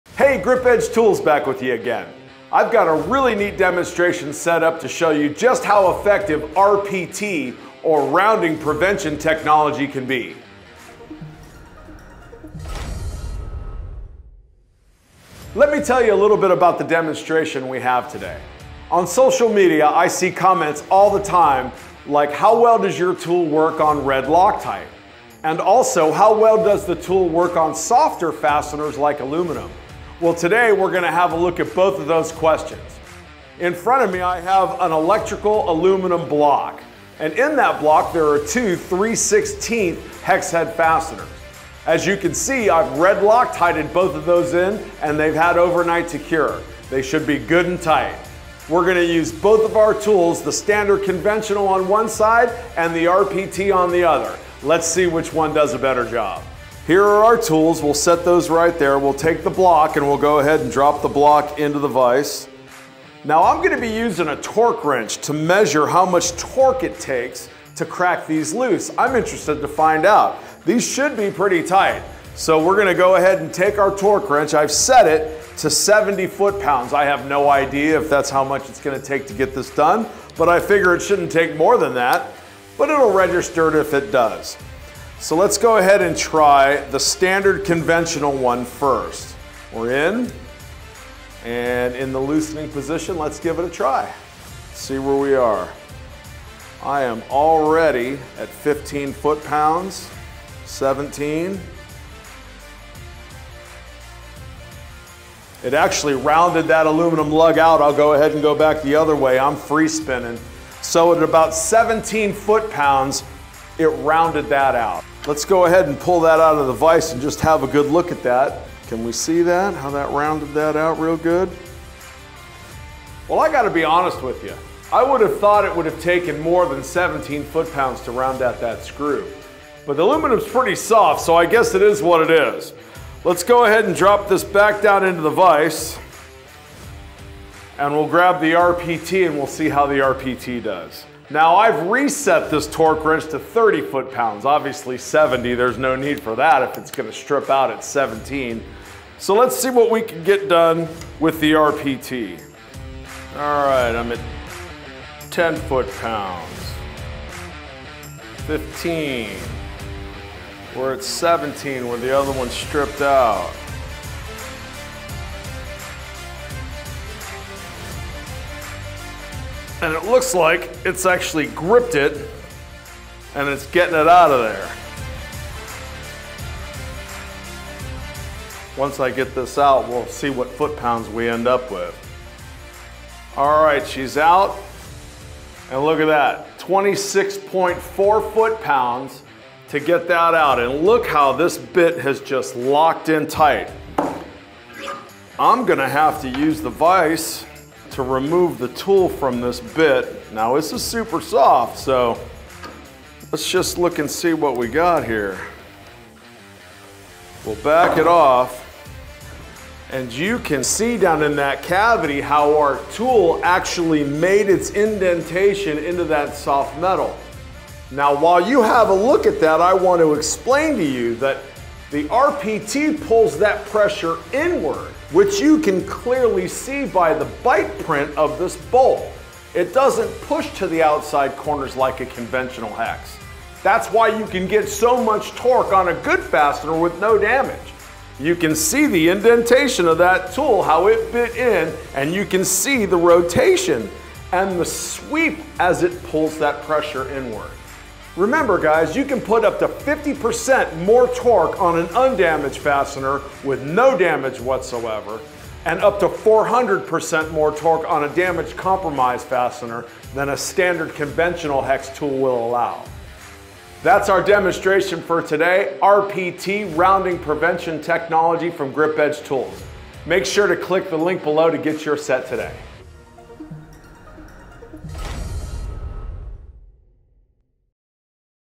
RPT Hex | Aluminum + Red Loctite | Torque Wrench Test | Extreme Use-Case Demo
Aluminum and other soft metal fasteners can be troublesome, especially if they're over-torqued. In this demonstration, Dan takes it to the extreme by applying 10 times the recommended amount of Red Loctite on an aluminum electrical fastener.

Both the standard and RPT hex bits are the same 3/16 size, but only ONE is able to successfully remove the aluminum lug. 💪

TIMECODES:
0:00 - Intro
0:15 - Demo Information
0:47 - Demo Setup and Additional Info
1:12 - Standard Hex Torque Test on Red Loctite
2:15 - RPT Hex Torque Test on Red Loctite
3:29 - Rounding Prevention Technology - How It Works